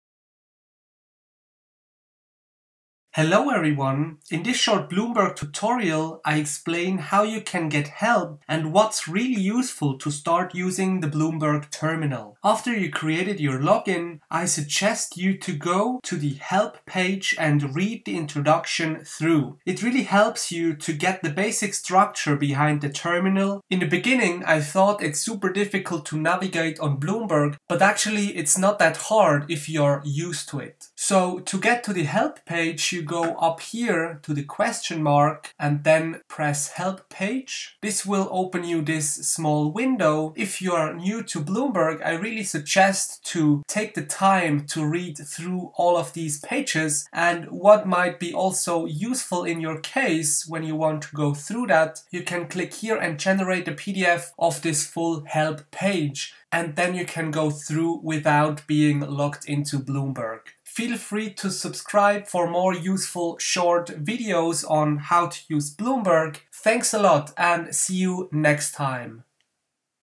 Help & getting started with the Bloomberg Terminal
This short tutorial explains how to simply get help and get started with the Bloomberg Terminal if you are new to it. In the beginning, the Terminal is not really user-friendly and it might look a bit unstructured. This is why I suggest news Bloomberg users first start with the "Getting started with the Bloomberg" documentation to get a good introduction.